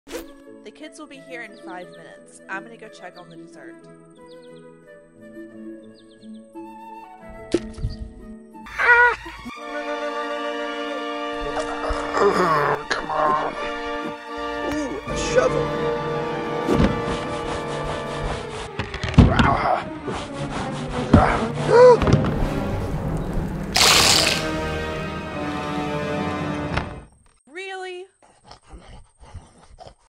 Dino Party - B
Father dresses up as a dinosaur for kids birthday party. He tries to get a snack before the kids show up.